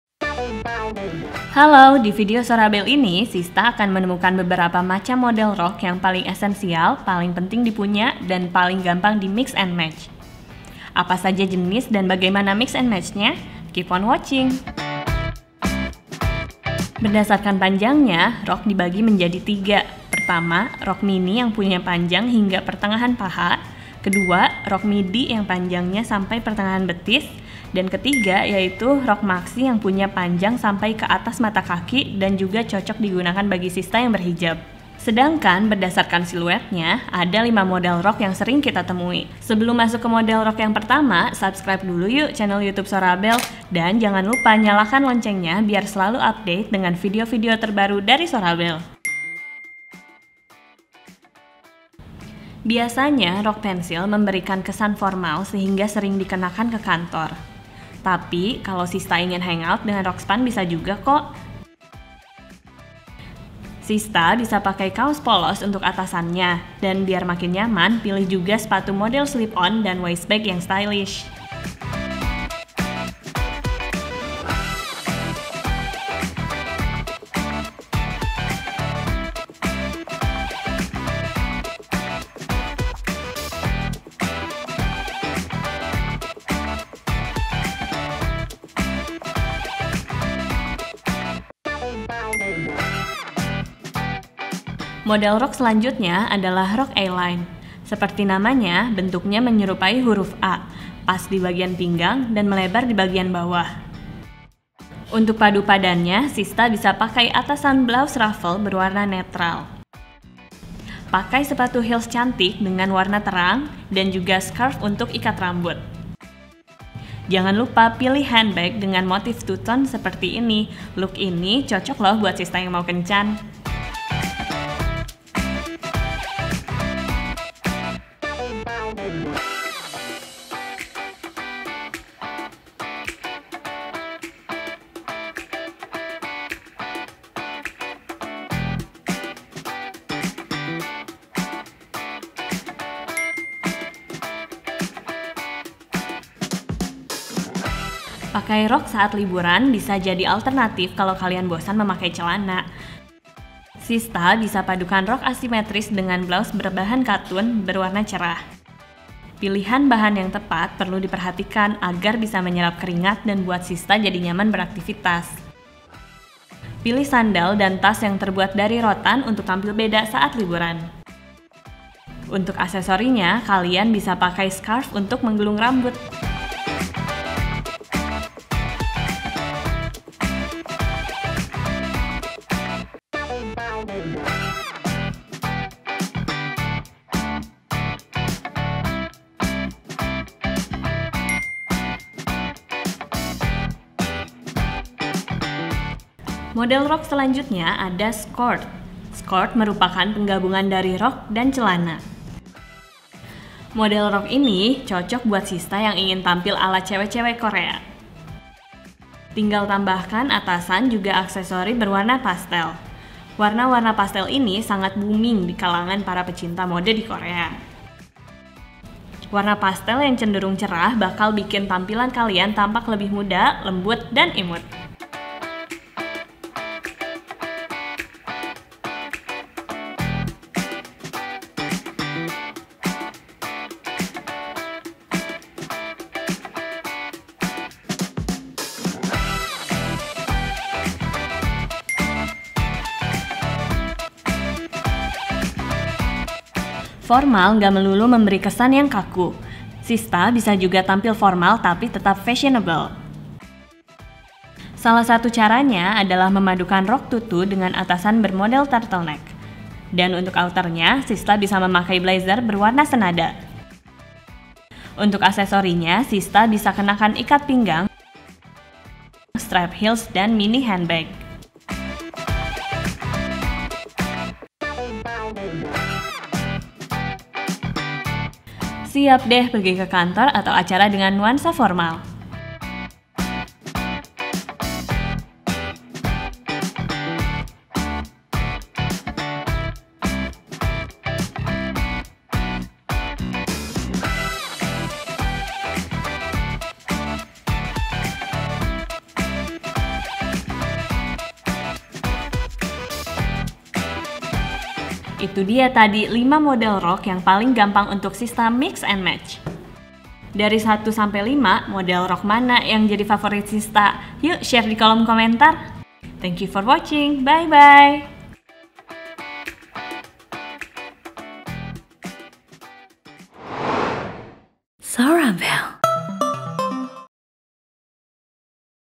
Wajib Punya! Model Rok Wanita yang Gampang Di-mix & Match | Sorabel
Model Rok Wanita yang Gampang di-mix & Match | Rok bisa jadi andalan Sista yang ingin tampil stylish. Nggak perlu bingung perlu pilih yang mana karena Sorabel punya 5 model rok cantik yang bisa Sista padu-padankan dengan fashion item lainnya. Tentunya, gampang banget buat Sista tiru!

Kalau mau dapetin produk yang ada di video, bisa langsung cek link di bawah ini:
1. Look #01
2. Look #02
3. Look #03
4. Look #04
5. Look #05

Temukan pilihan gayamu sekarang di Sorabel karena di Sorabel bisa Coba Dulu Baru Bayar! Jadi Sista makin yakin deh sama gaya kalian.